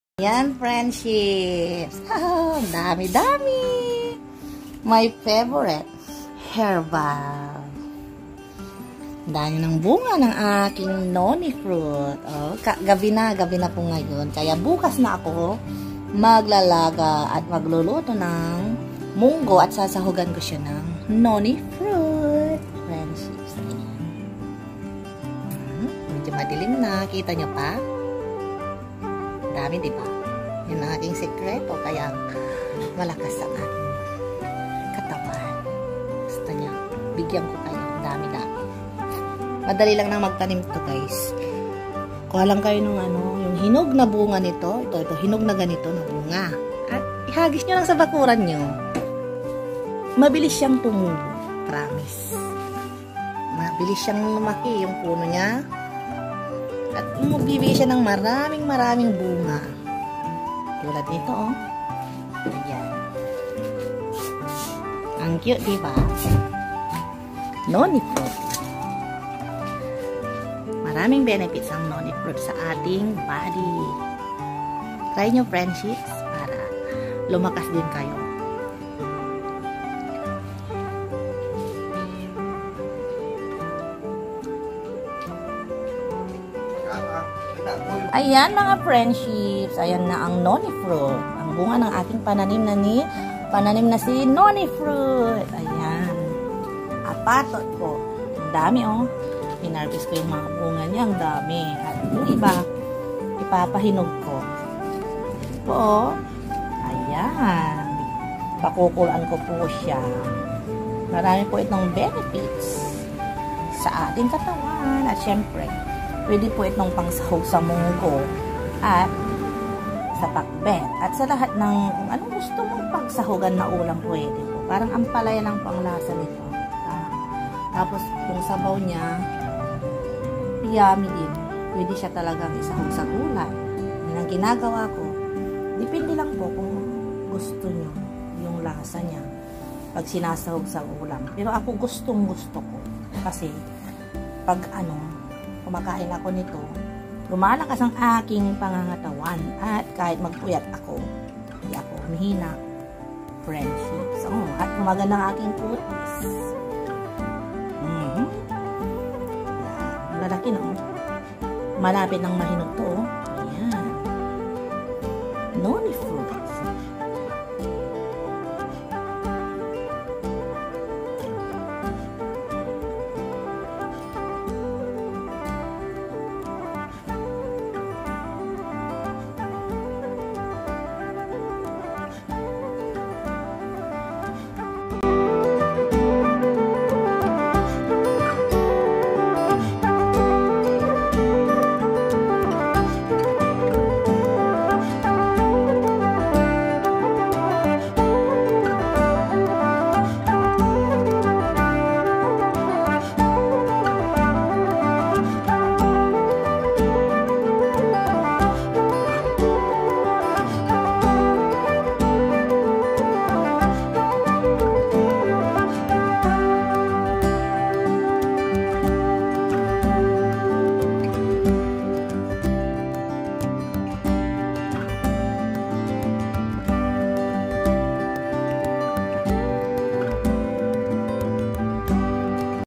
NONI FRUIT or APATOT ,wow angdami dami ulit bunga friendships
Maraming pwedeng gamotin
ang prutas na ito sa ating katawan 💝
Miracle fruits 💖💞💕
NONI FRUIT 💖
Healthy life Happy life
BLESSINGS 🥰